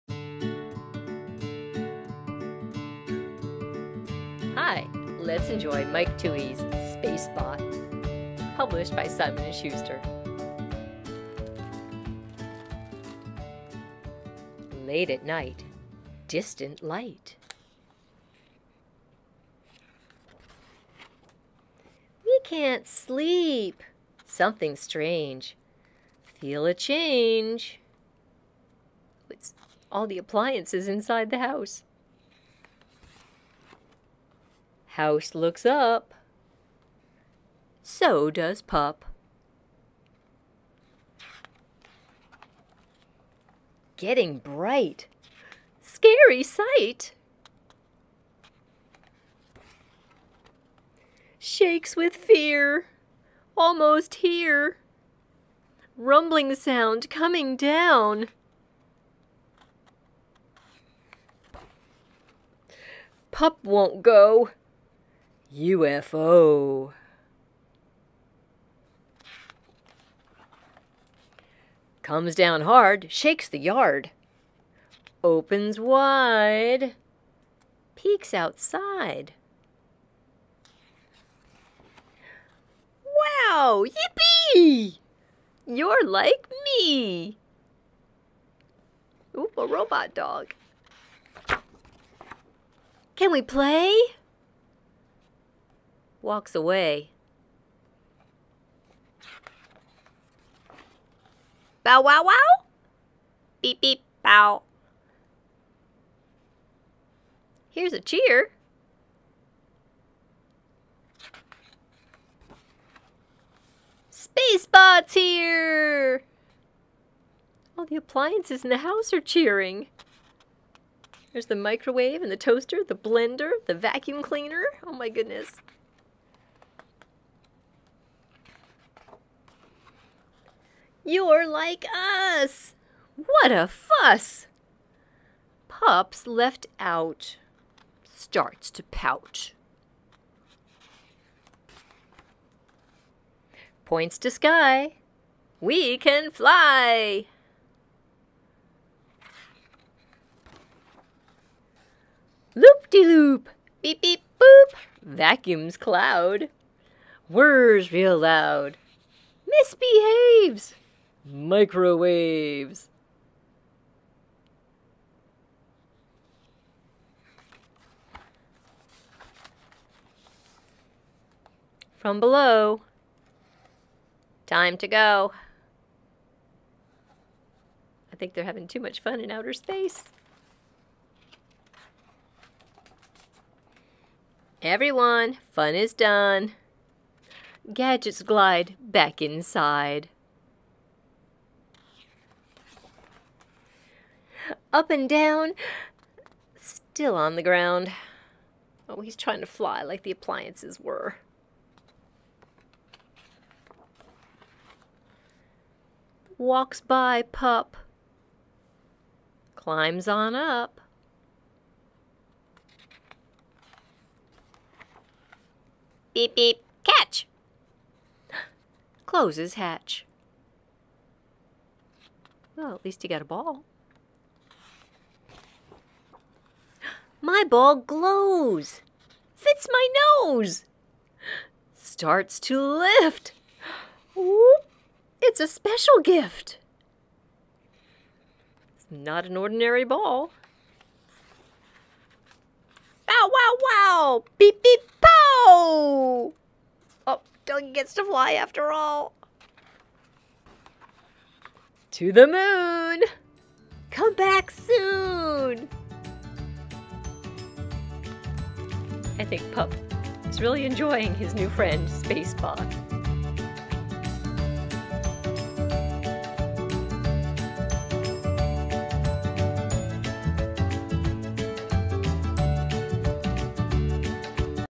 Spacebot
Miss Janet reads "Spacebot" by Mike Twohy with permission from Simon & Schuster Books.